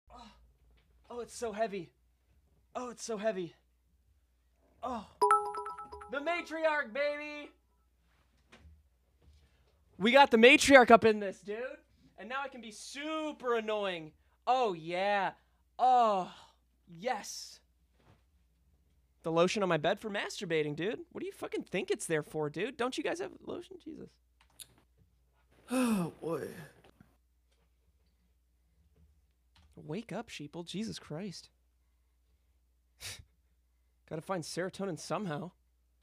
Eagle Eye Viewer Catches Something In Steven Suptic Background
This clip was taken from Steve's Stream, check him out